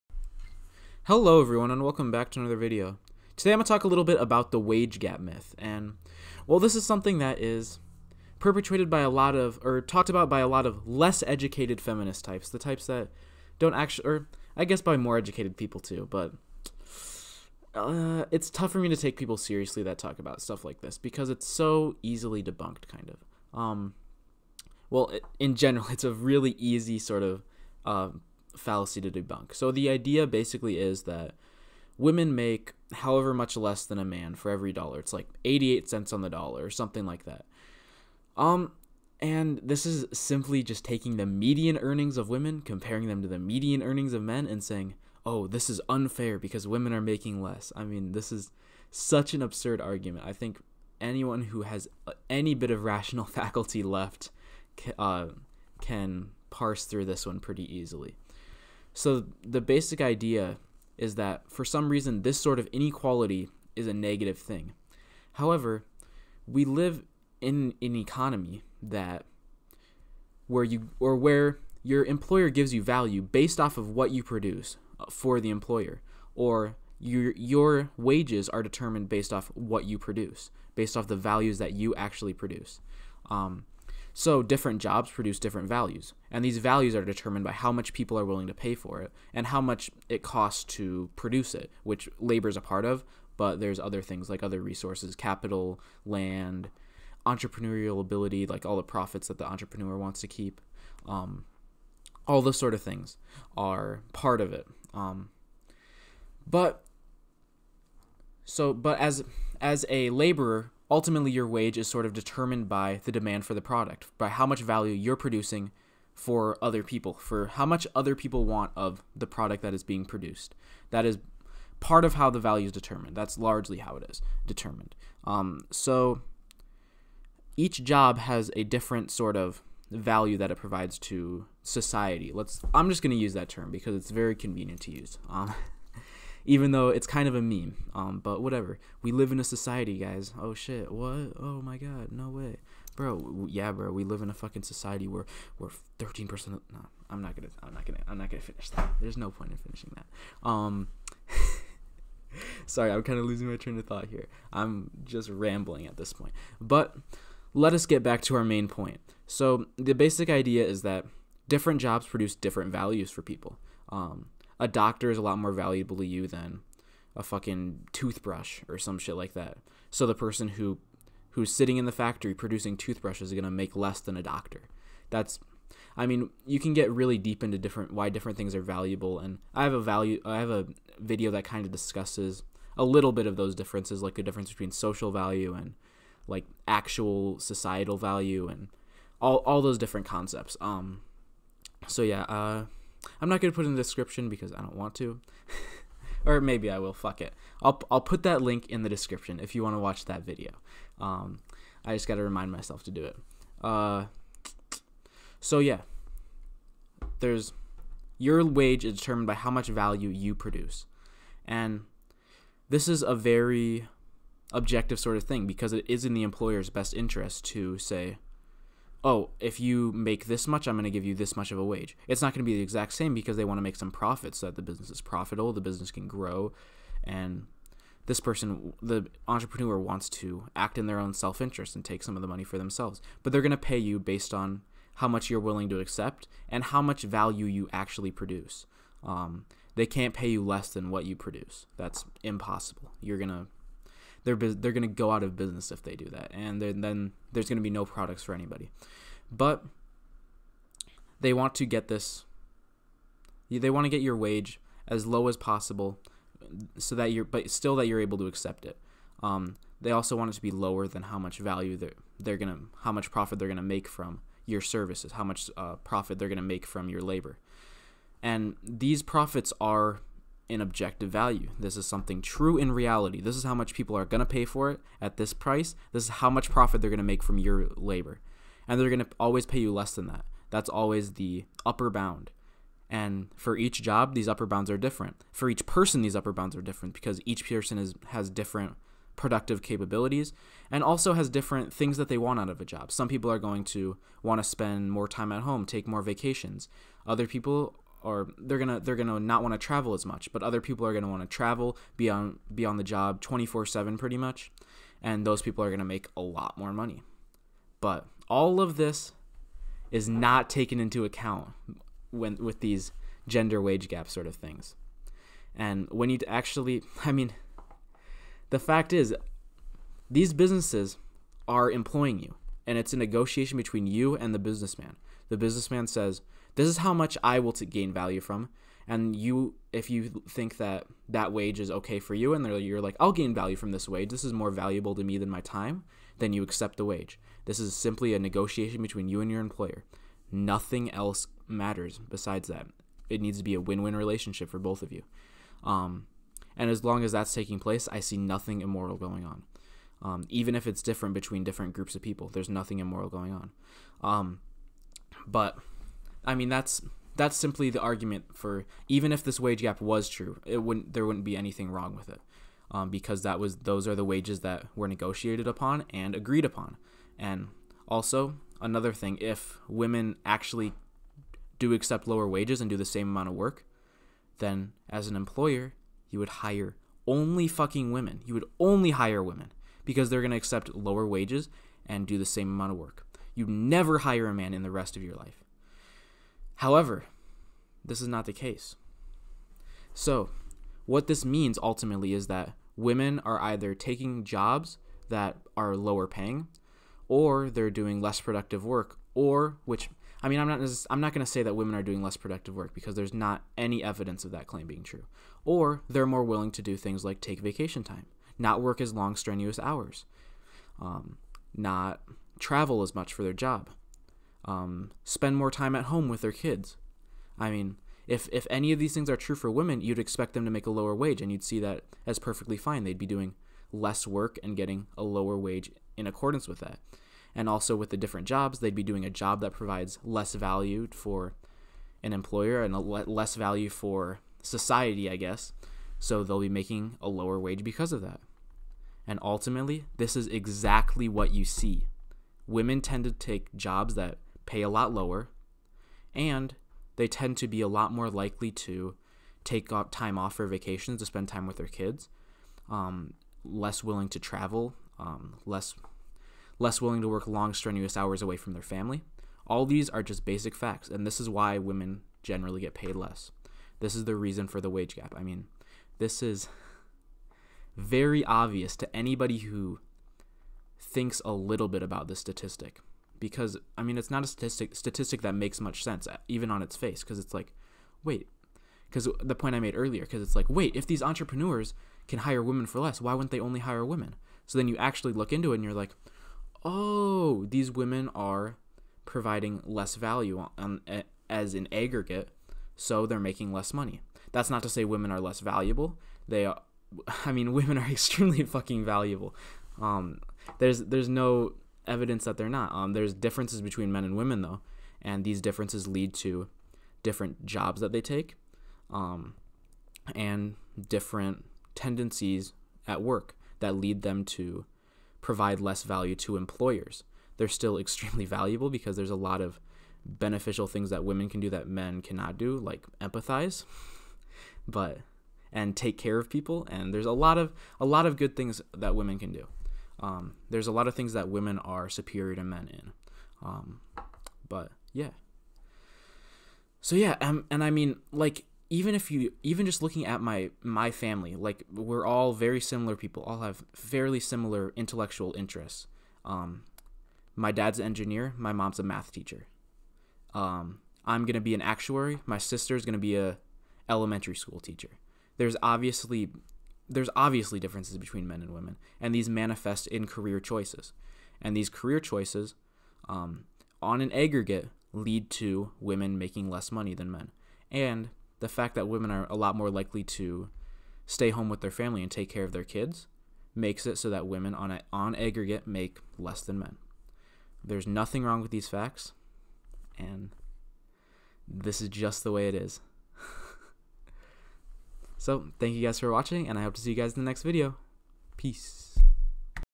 The Wage Gap Myth
In this video, I talk about the false statistic that women make less on average for men for the same work.